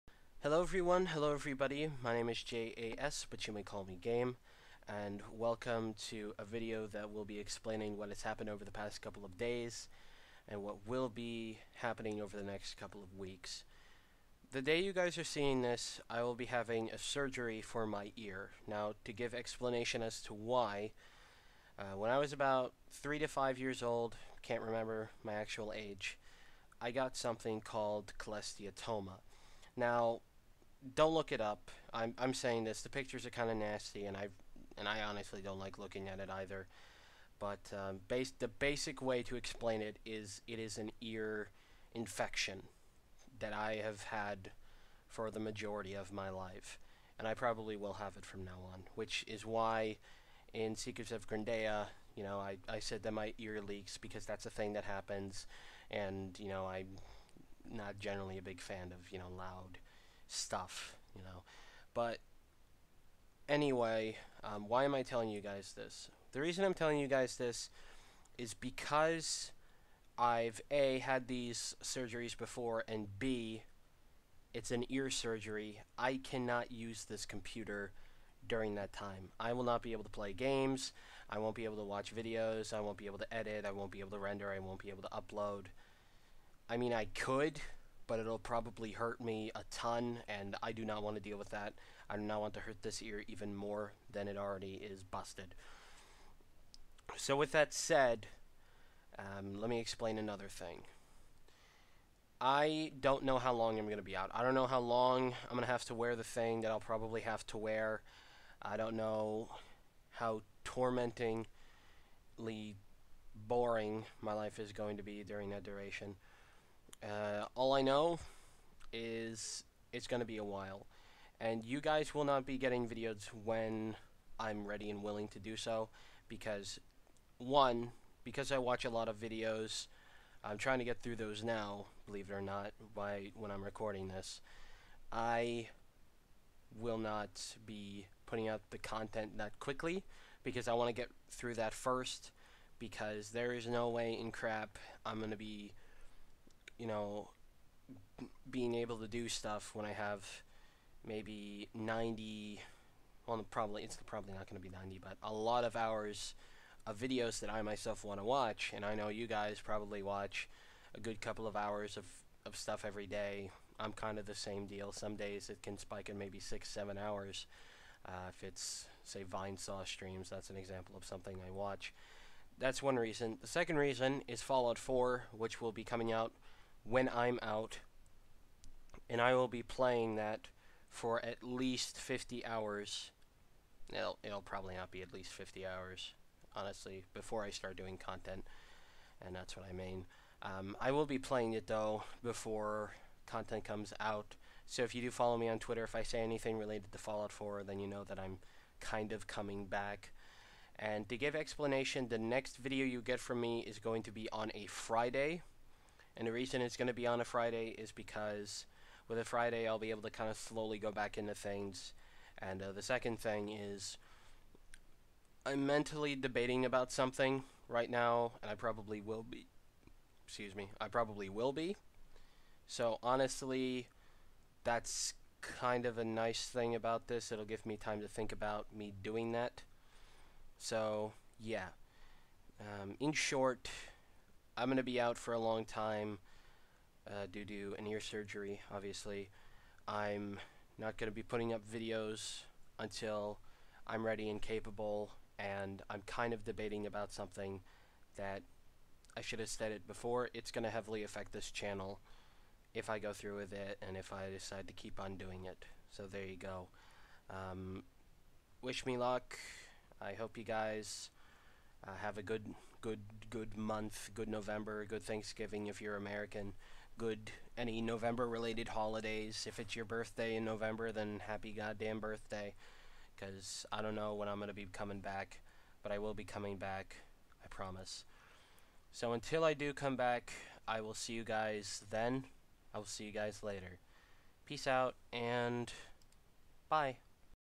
Hiatus beginning today. Unknown ending date. Explanation inside.
Couldn't think of anything to go with what was said, so that's why everything is the way it is for the video.

To sum things up:
-I have to go have an ear surgery today due to cholesteatoma that I've had since I was young.
-Because of said ear surgery, I can't do anything computer wise, from playing games to recording.
-I won't be back even when I'm  fully able to due to a mix of videos I wish to watch and wanting to play Fallout 4 since it comes out a week before surgery.
-I'm debating about something that will be finalized when I'm fully capable of returning.